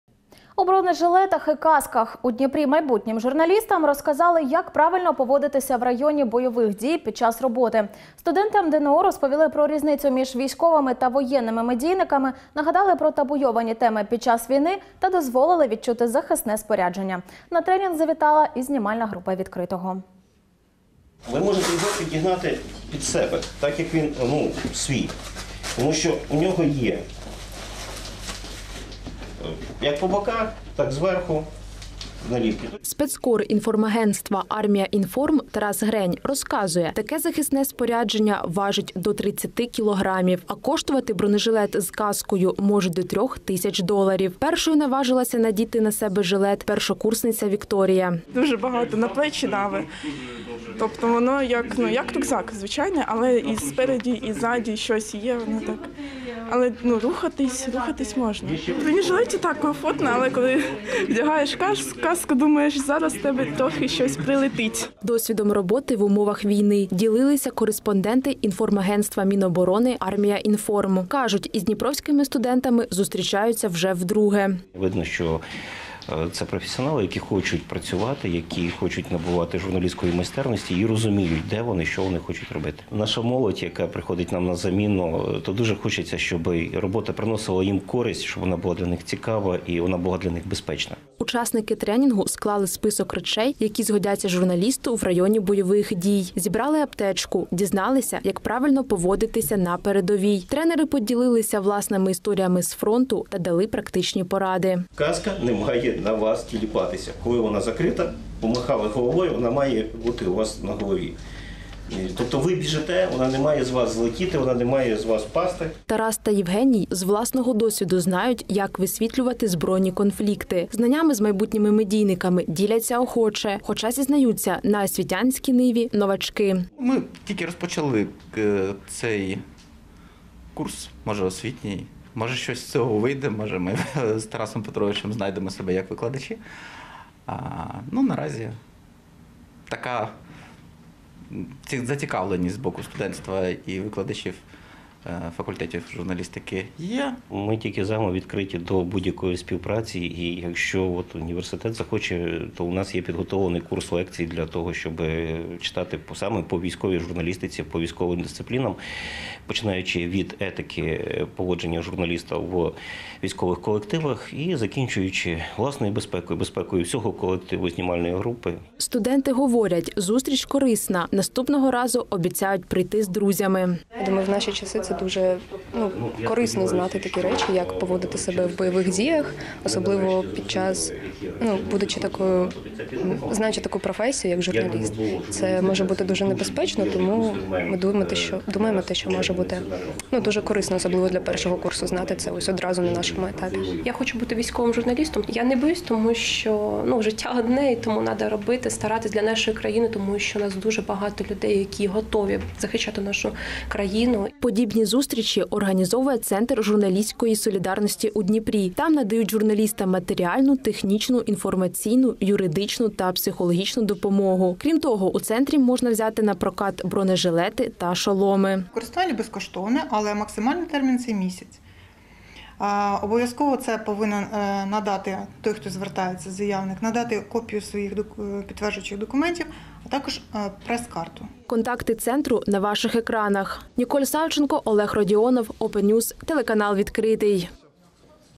У Дніпрі майбутнім журналістам розказали, як убезпечити себе в районі бойових дій
У бронежилетах і касках. У Дніпрі майбутнім журналістам розказали, як правильно поводитися в районі бойових дій під час роботи. Студентам ДНУ розповіли про різницю між військовими та воєнними медійниками, нагадали про табуйовані теми під час війни та дозволили відчути захисне спорядження. На тренінг завітала і знімальна група Відкритого.